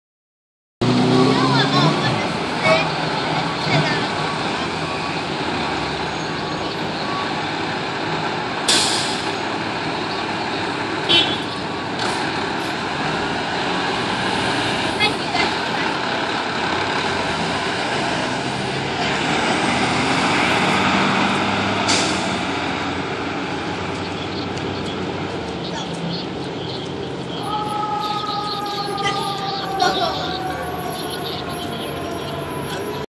Fdny Ladder 47
Responding to a stuck elevator.

Location Bronx,NY